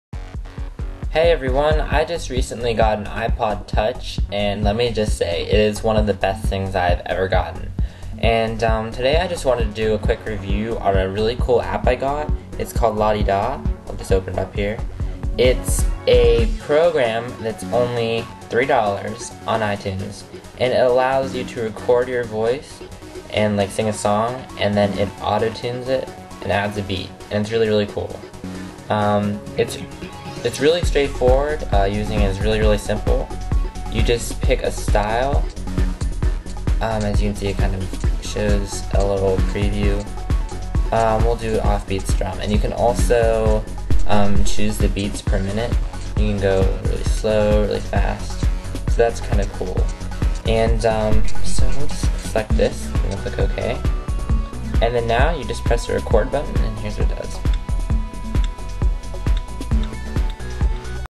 LaDiDa App Review (iPod Touch 4)
Quick review of a cool app called LaDiDa.  Allows you to record a song, add autotune and background music.

Only $2.99 in the app store.  It's worth the money.